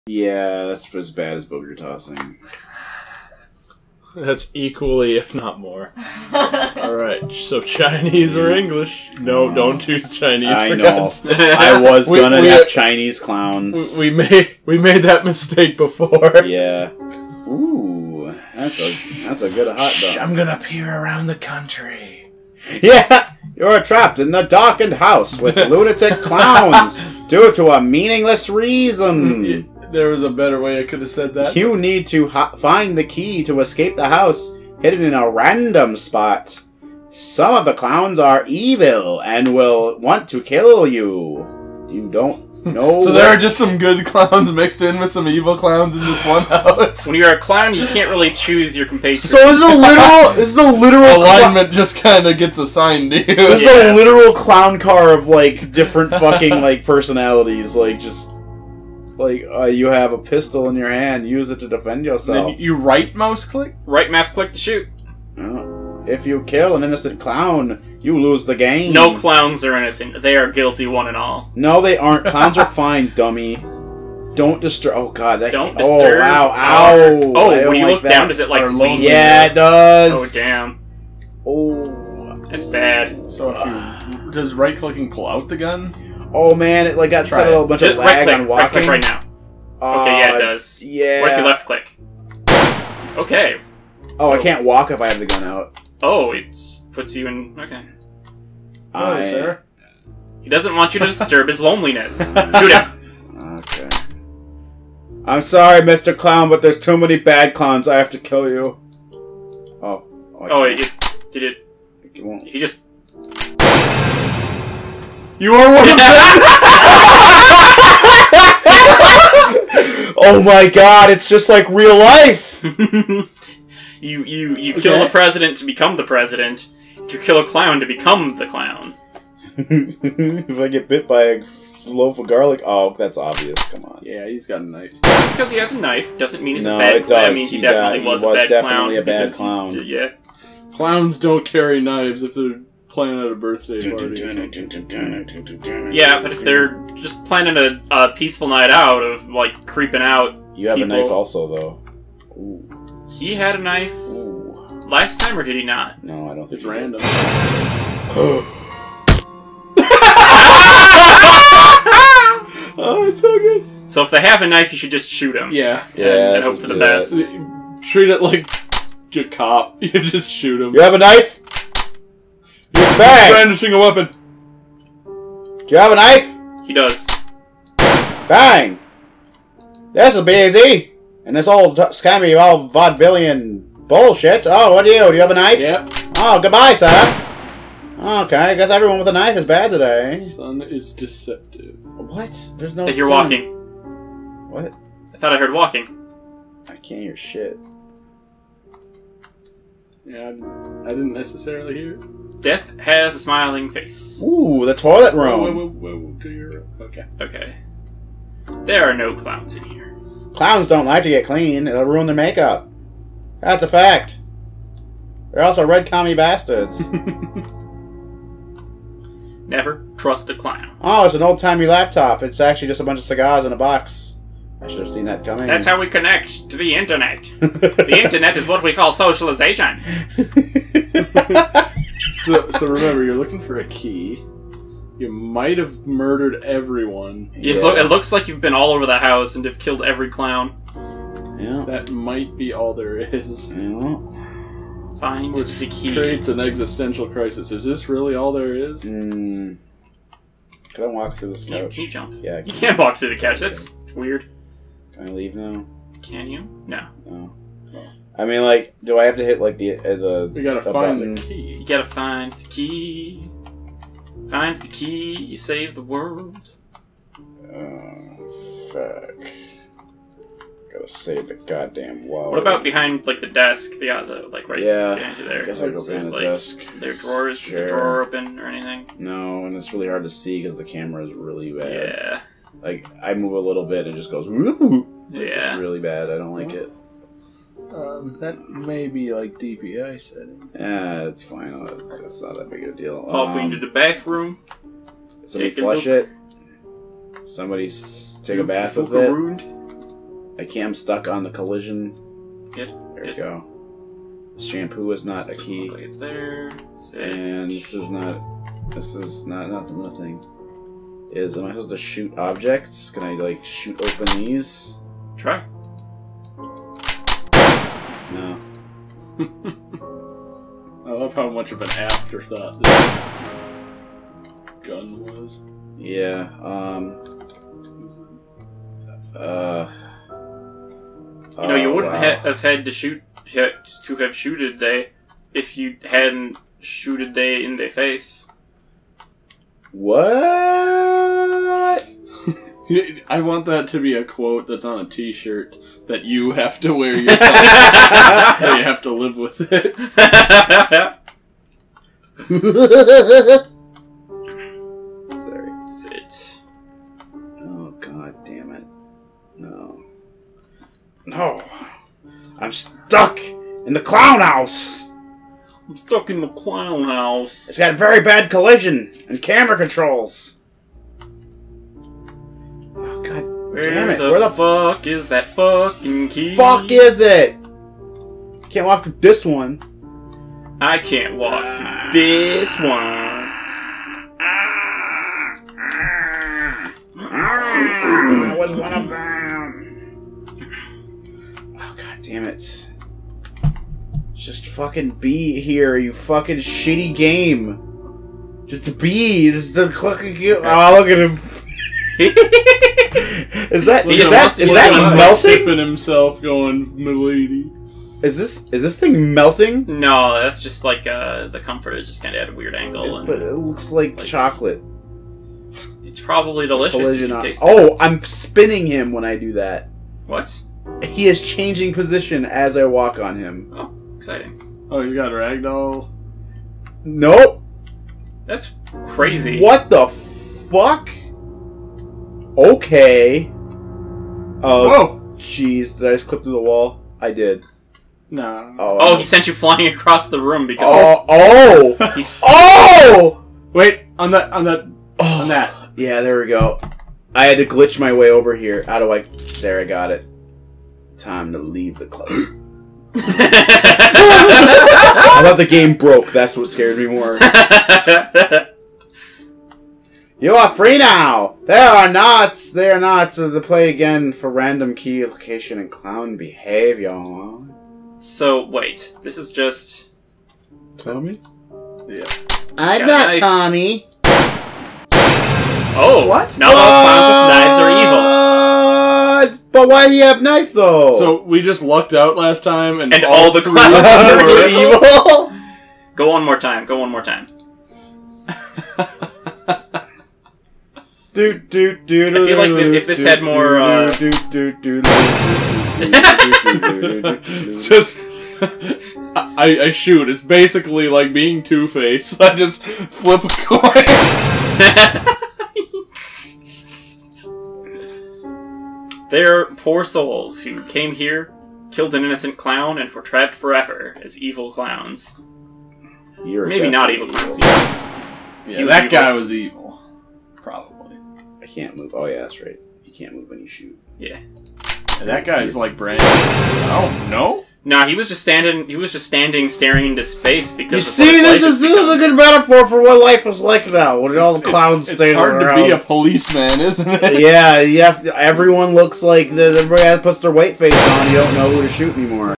Basement Quality TFTB2016 - Clown House
Pat, Nathan, and Taha have to make snap judgement of a clown's character before they kill us.

Make the wrong call and you'll be forced into Clowndom